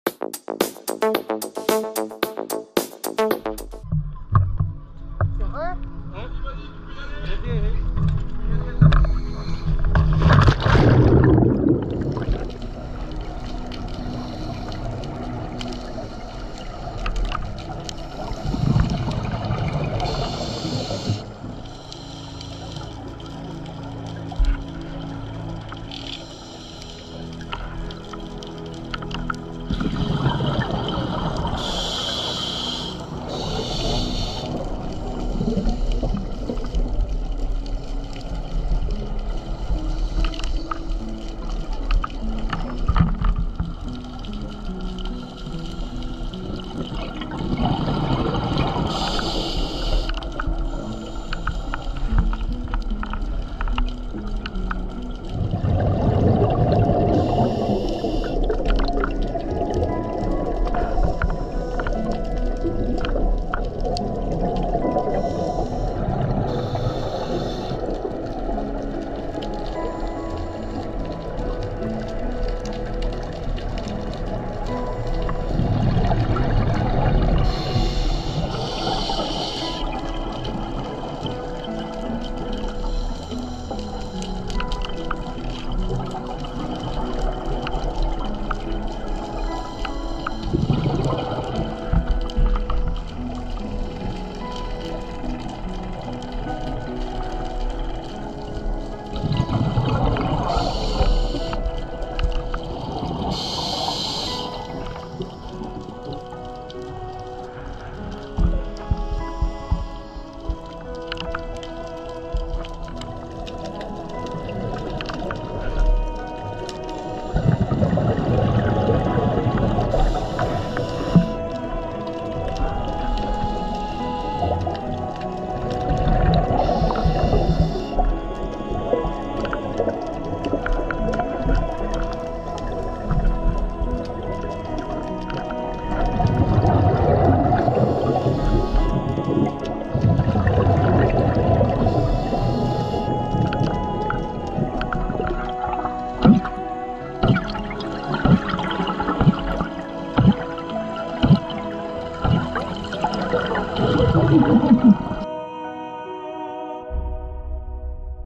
La Gabinière Est
Plongée sur la pointe Est de la Gabinière.
Profondeur max : 32.3m
Durée : 48min
Deco 3min/Safety

Des mérous, forcément... mais surtout des dentis par dizaines, des spiros, une énorme araignée de mer...